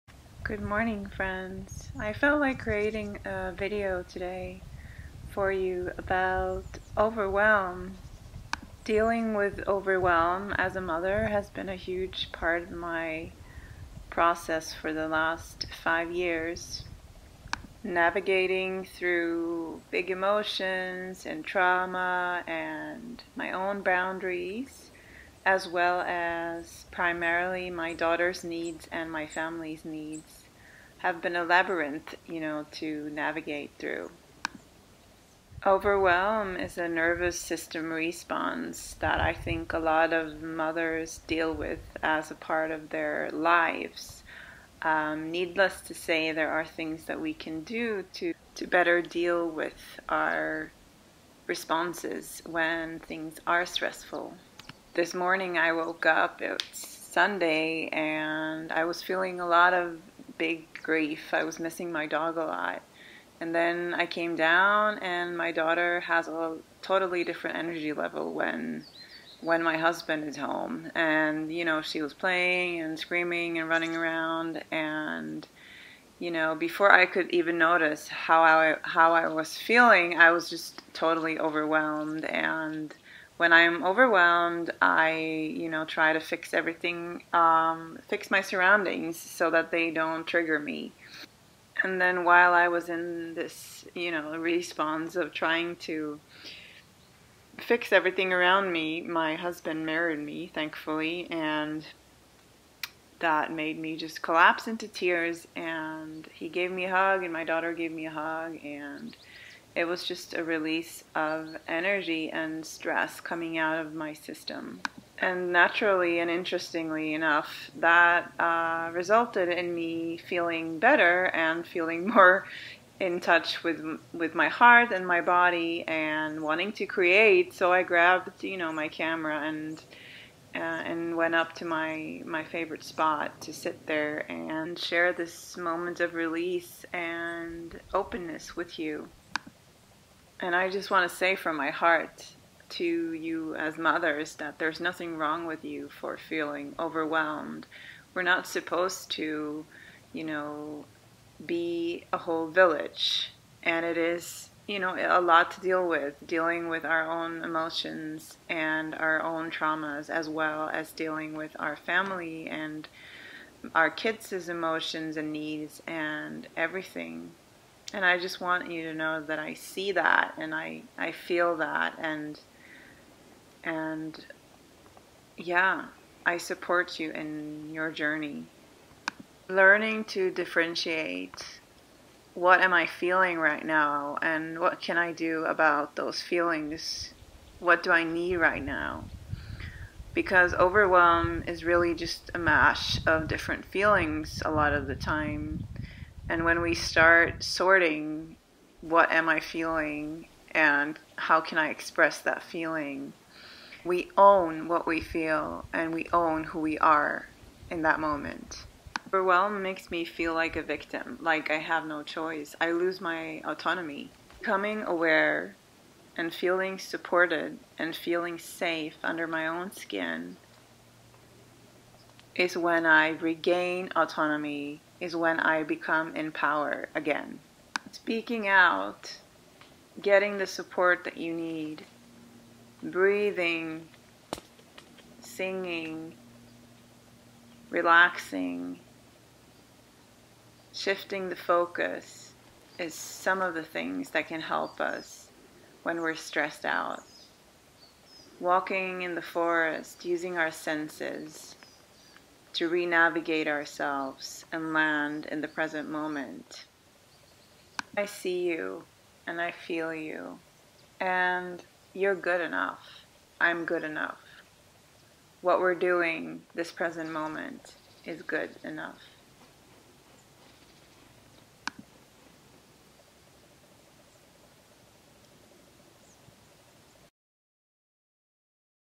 A morning of overwhelm
In this video I share about feeling overwhelmed. This is a feeling that has been with me most of my life, but definetly reaching a peak, after becoming a mother. Overwhelm is a part of motherhood for me, and also a journey from victomhood- into Autonomy. This is me sharing about the feeling, what it is for me, and what I do to return to balance.